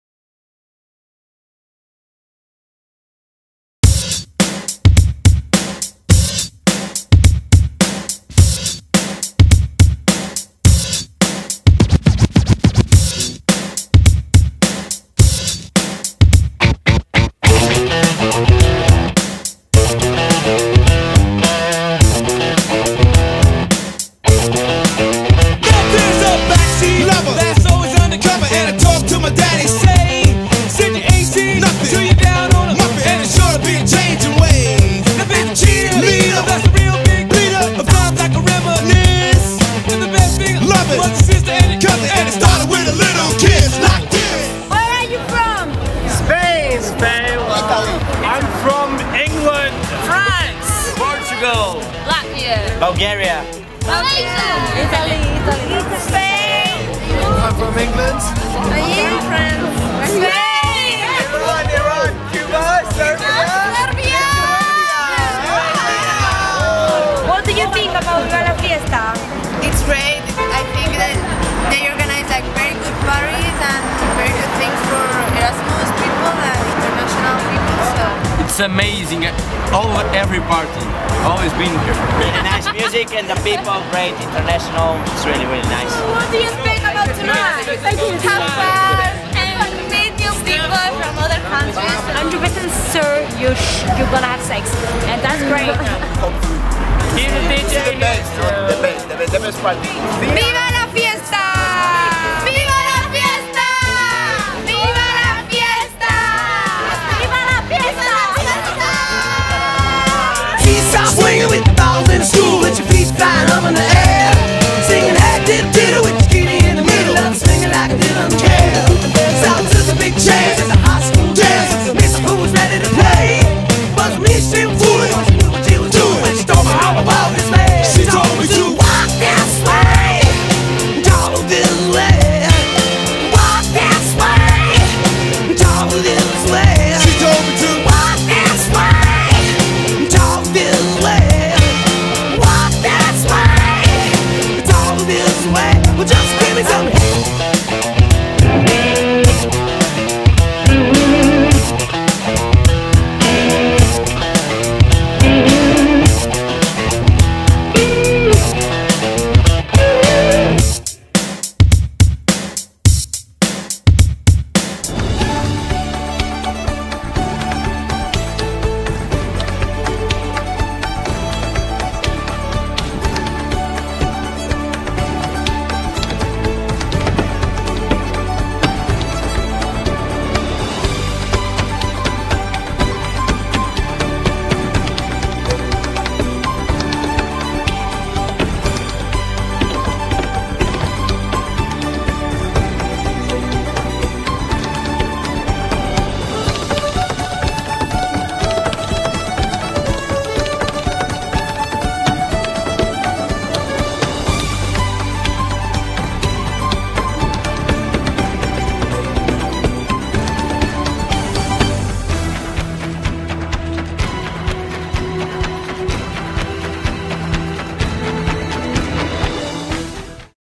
LEEDS (VII) - Eighties (80s) v Nineties (90s)
Viva La Fiesta introduced Leeds to a battle of the glorious decades - 80s v 90s. Style, fashion, music ... great night @ The Cockpit. LEEDS. LIVE IT. LOVE IT. Vivaaaaaaaa :)